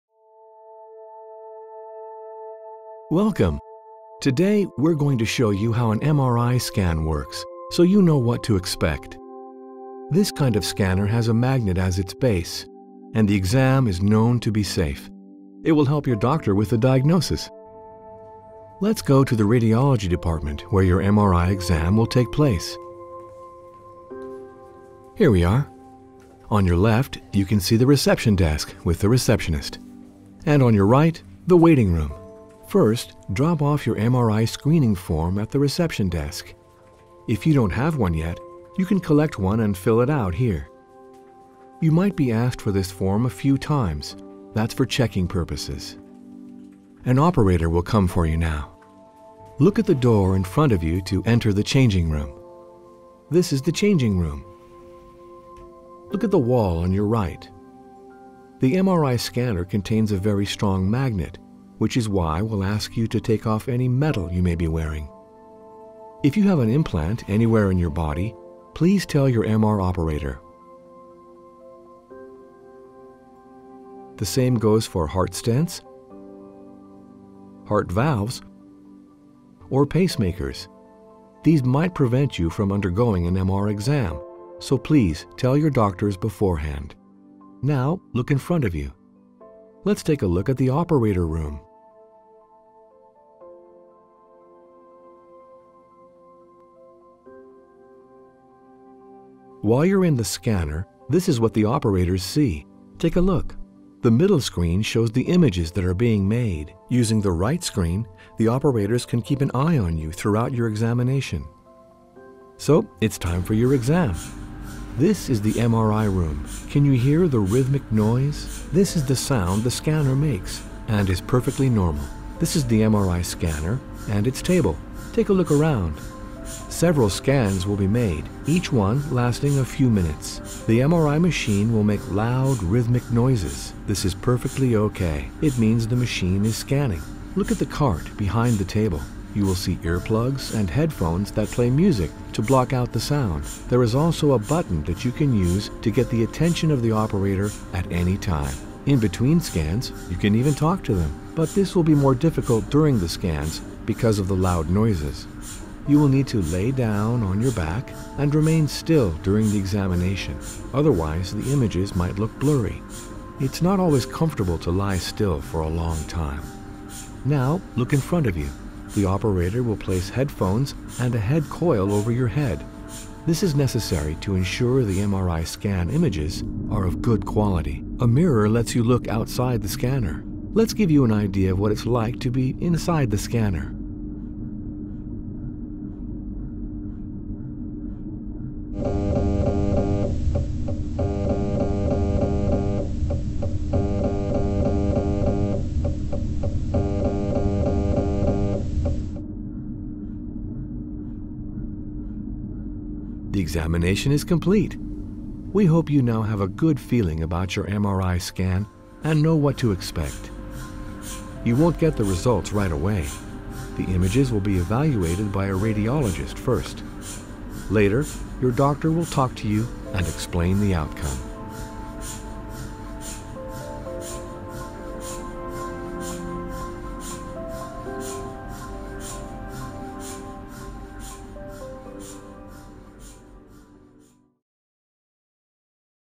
How does an MRI scan work? - in Virtual Reality
Before anything else, preparation is the key to success.”

- Alexander Graham Bell

Knowing what to expect before an MRI scan can be an important part of a positive patient experience. Patients who are well prepared are likely to be calmer, which helps scans to run smoothly and lets your staff work more efficiently. Both patient and staff satisfaction play a role in your institution’s economic well being.

 We have recognized the value of good preparation. Using virtual reality, we have created an experience that takes your patients on a journey – not just telling them but showing them what to expect. Together with Infor-Med, Maxima Medical Center Veldhoven and Radboud University Medical Center Nijmegen, we’re bringing MRI to life and helping enhance the patient experience.